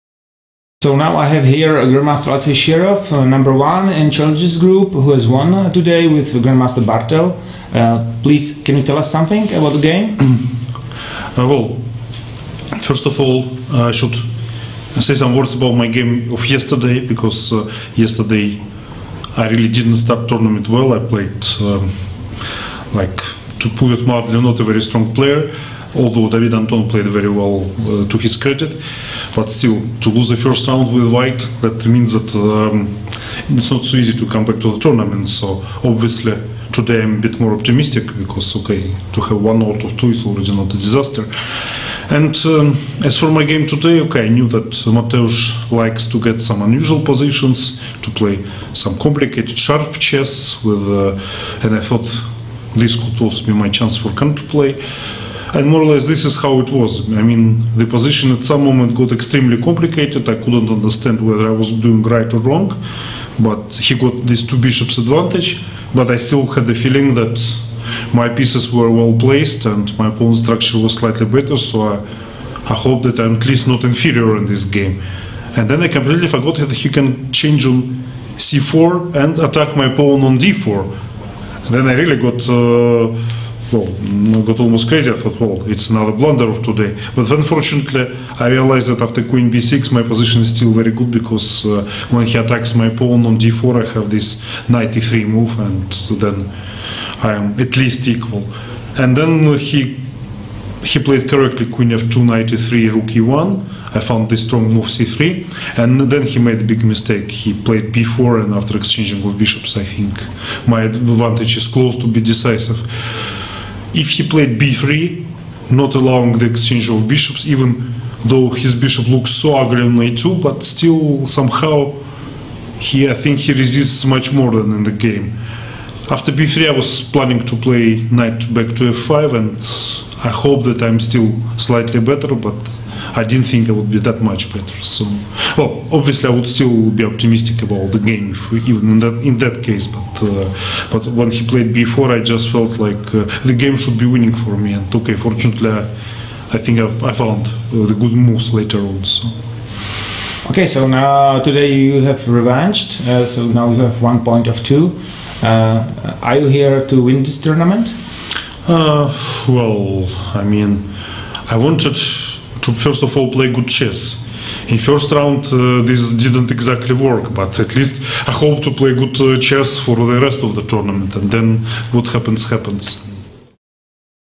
Interview with Alexei Shirov after his victory in Round 2
Prague International Chess Festival
Mateusz Bartel - Alexei Shirov 0-1